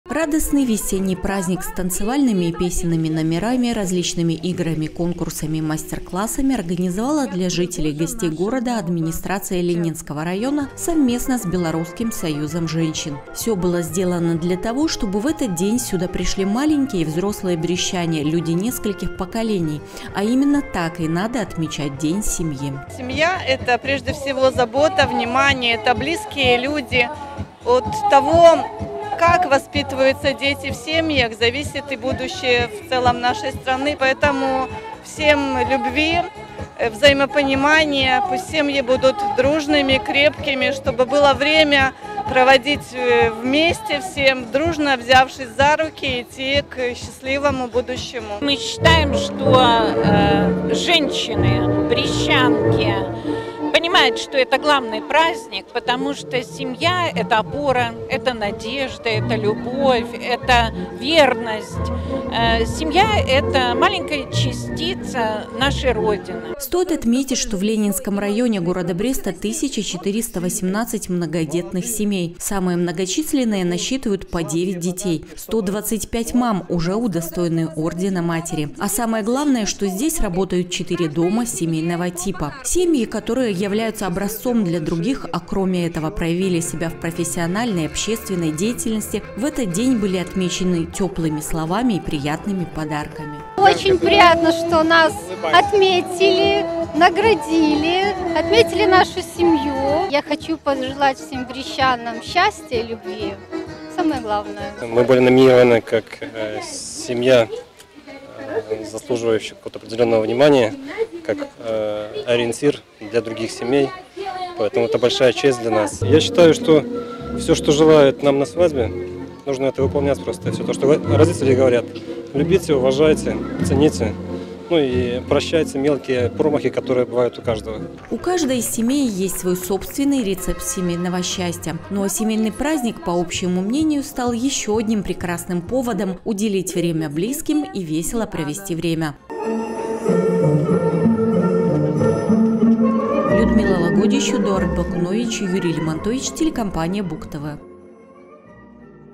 2022-05-17 г. Брест. Праздник «У Причала». Новости на Буг-ТВ.
Наш город открыт ко всему прогрессивному и новому, но при этом неустанно чтит свои традиции. Одна из них много лет назад зародилась в Ленинском районе и приурочена ко Дню семьи. Праздник " У причала" вновь собрал многочисленных гостей и жителей, здесь же прошло чествование лучших семей, которых поздравили  теплыми словами и подарками.